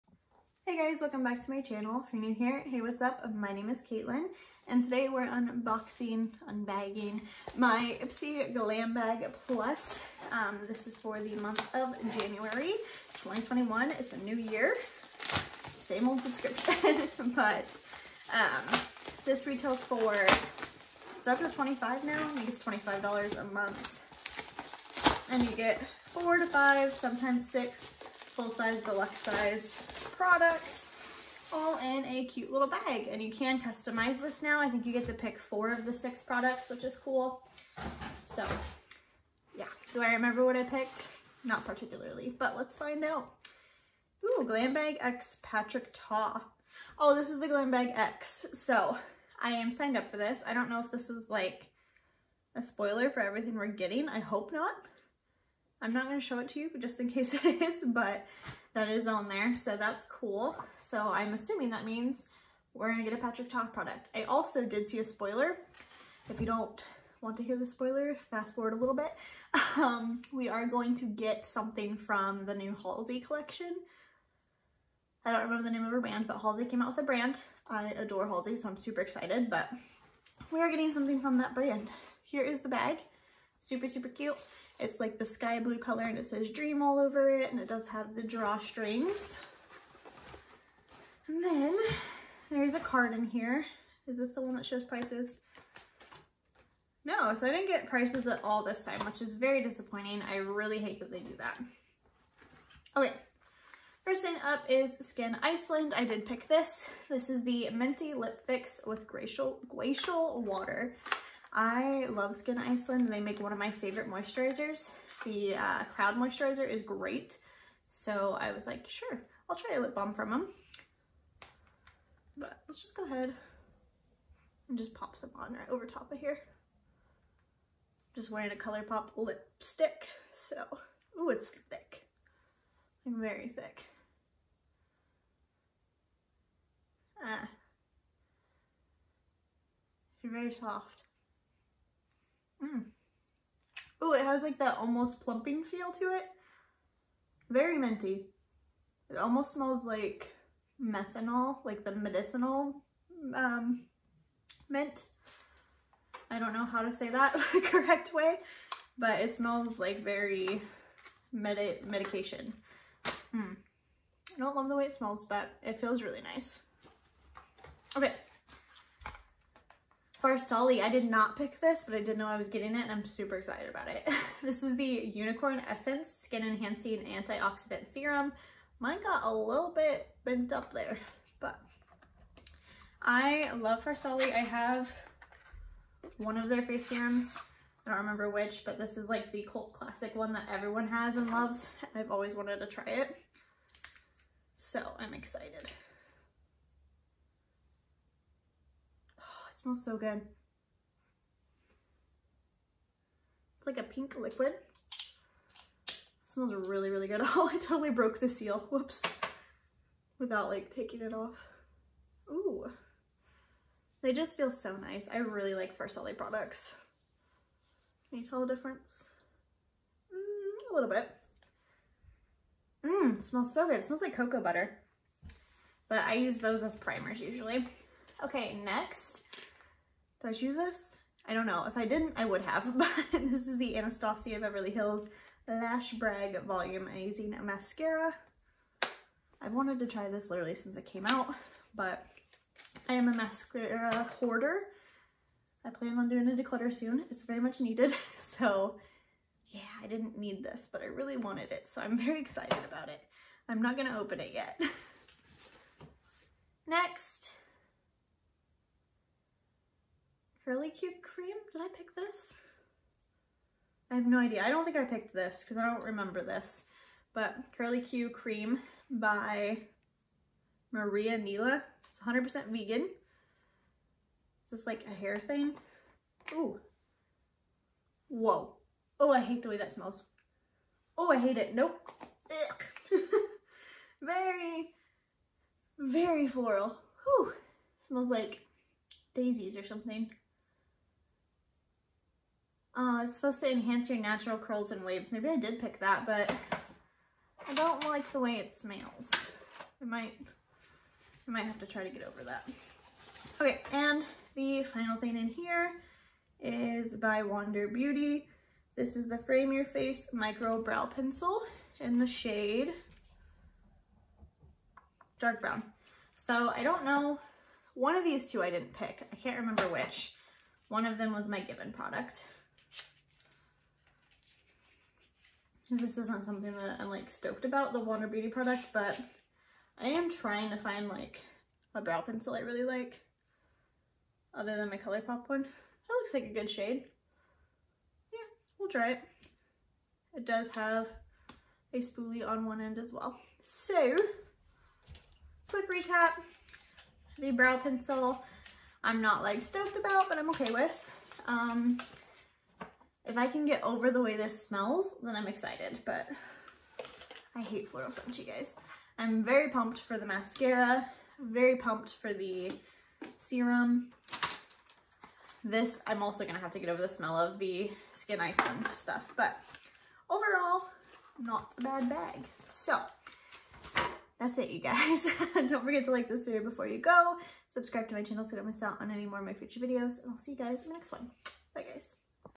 Ipsy Glam Bag Plus Unbagging--January 2021!
Have unwanted makeup or other items? Sale them using Mercari.! Sign up using my code: DKQRPA

Products Mentioned:

Skyn Iceland Lip
Mint not available. $16. Cruelty Free.

$54. Cruelty Free.

$25. Cruelty Free.

$33. Cruelty Free.

$21. Cruelty Free.

Total Value: $149!